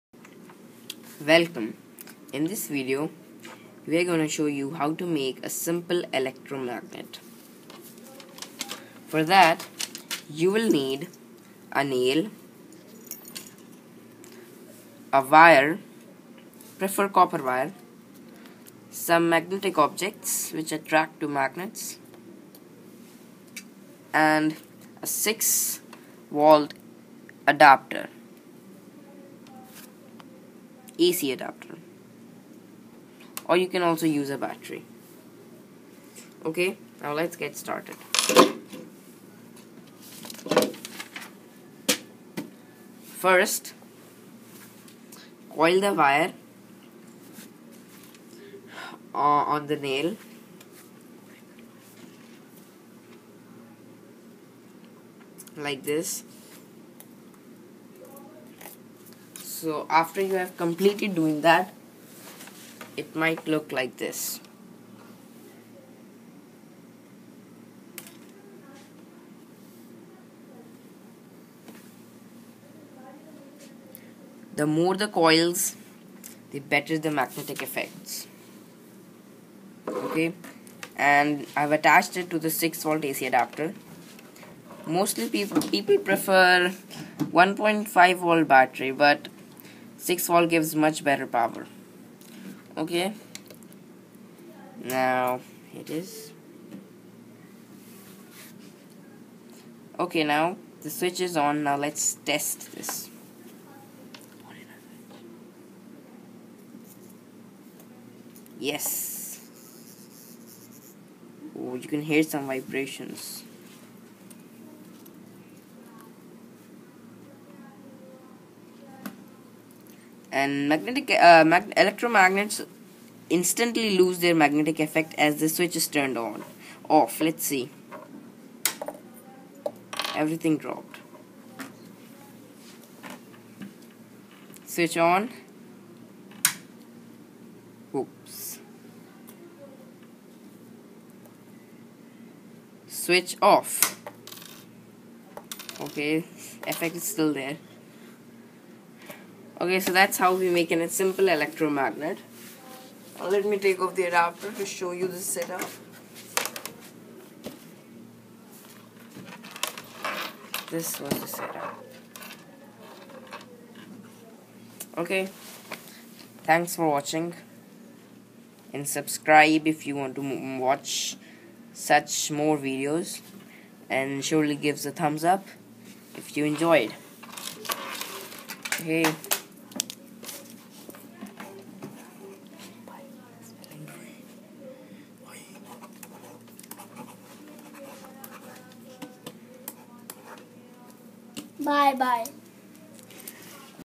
How to make a simple electro-magnet!
This working model of an electro-magnet clearly shows the magnetic effect of electric current. The things you need,how you need to do it,its all here !